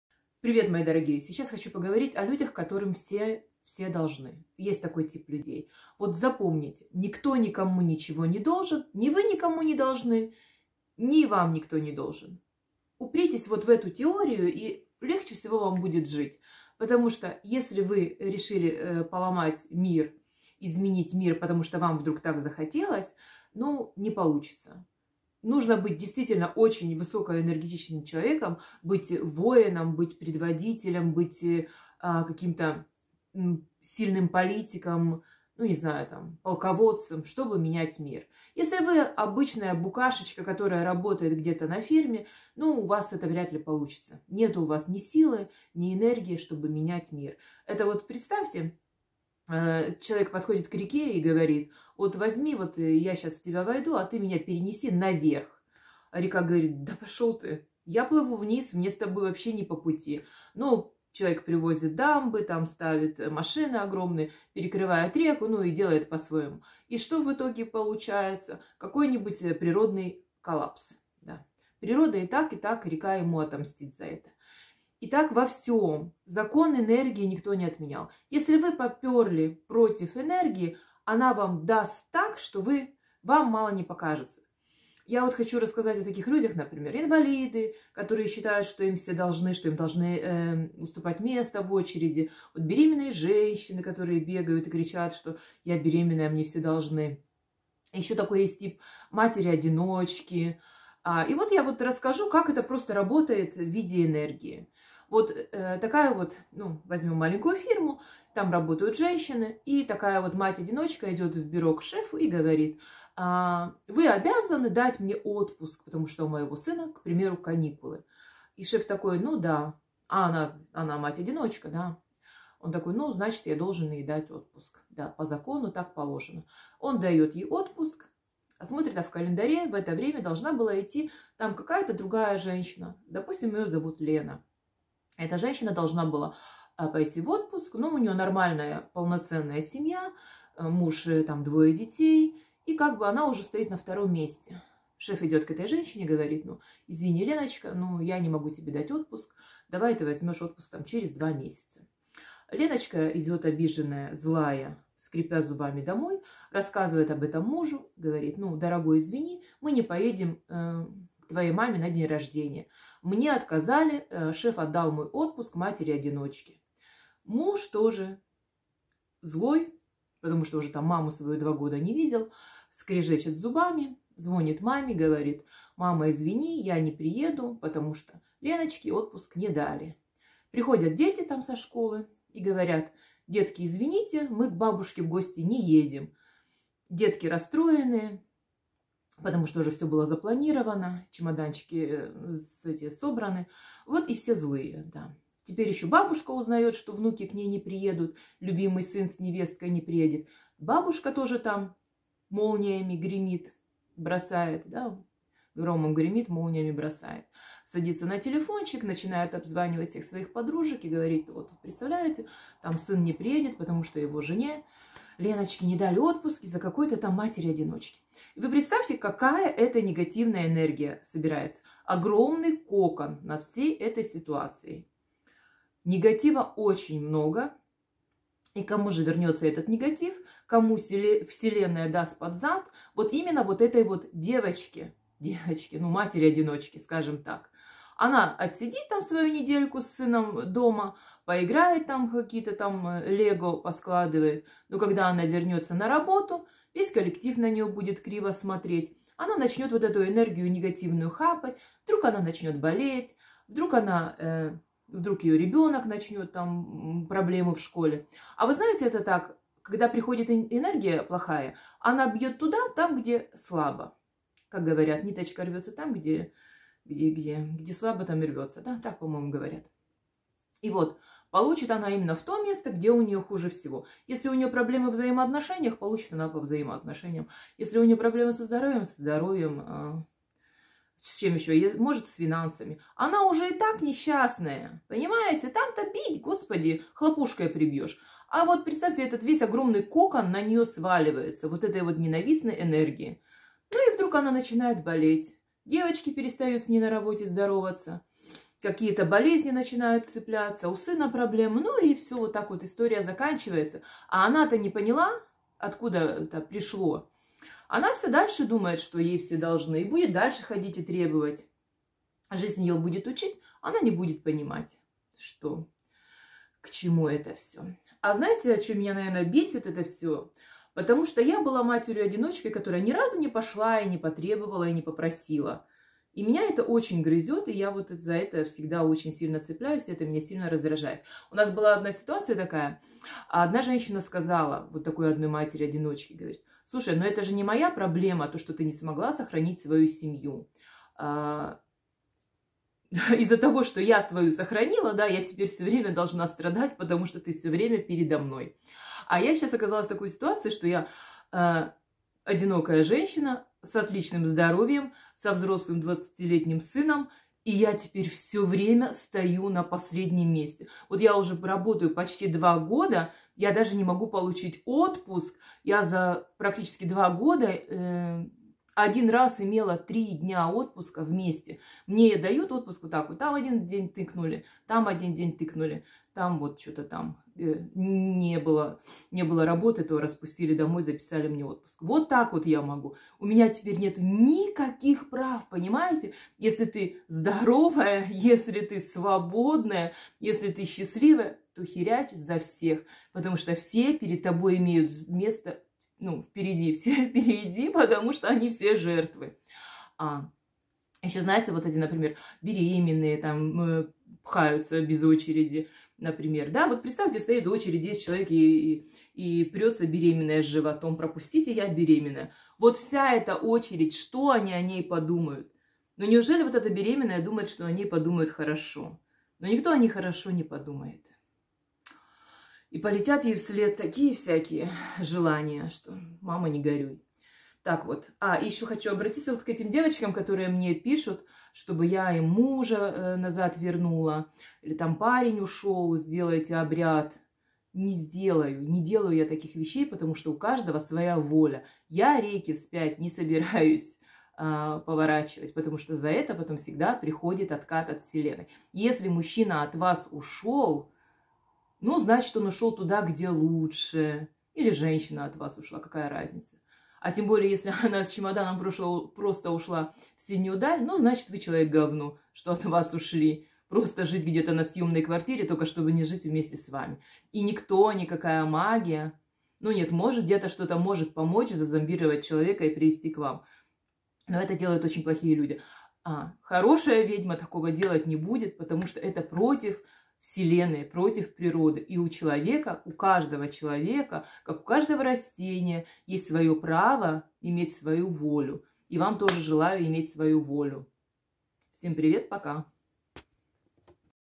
"Мне все должны". Стоит ли плыть против течения.
У Вселенной свои законы, закон энергии никто не отменял. Никто никому ничего не должен.
Счёт в банке: Natalia Paszkowska DE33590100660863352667